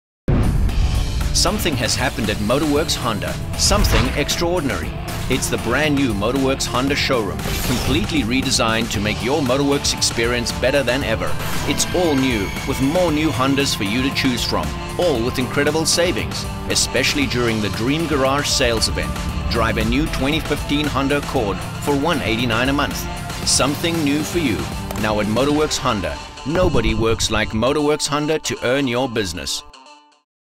The BRAND NEW Honda Motor Werks Showroom is Open!
We're so excited that the Motor Werks Honda showroom is open--completely new and redesigned! All with incredible savings during the dream garage sales event! today!